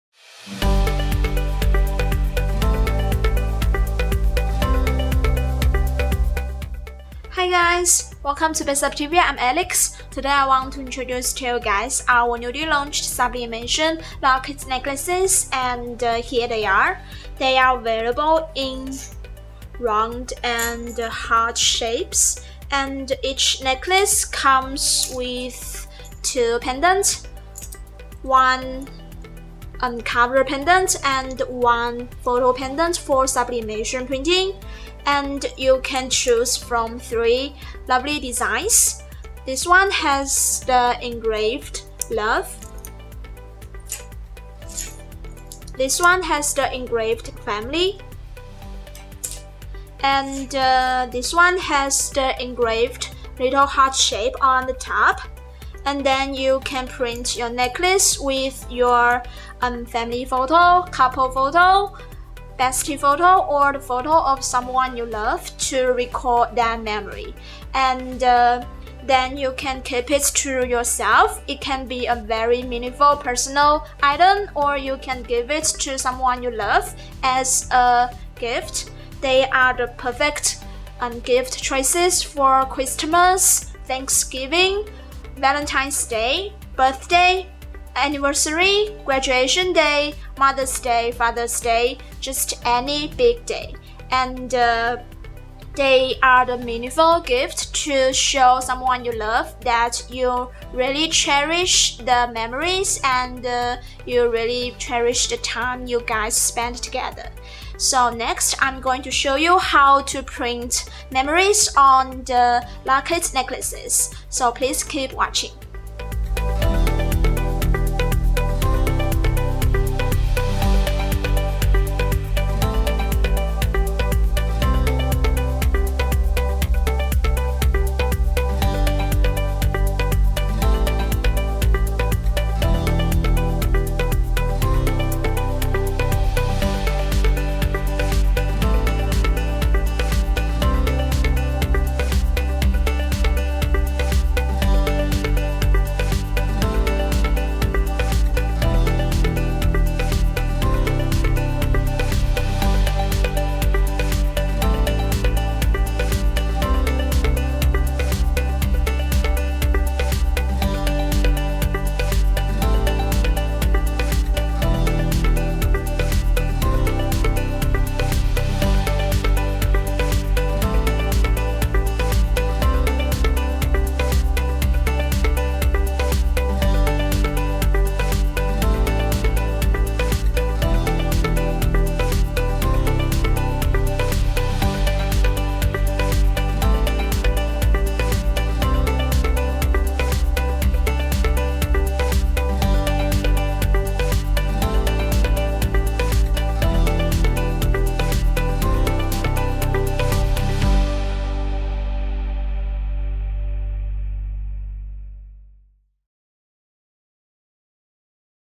How to Print Sublimation Photo Locket Necklaces ? DIY Photo Memorial Locket Necklaces
BestSub just launched some sublimation photo locket necklaces! Each comes with one cover pendant and one sublimatable photo pendant. You may choose from round and heart-shaped necklaces and three lovely engraved designs—“FAMILY”, “LOVE” and a little heart shape.

They’re unique gifts for your loved one on Christmas, Valentine’s Day, Thanksgiving, birthday, graduation day, anniversary, Mother's Day, Father's Day etc.!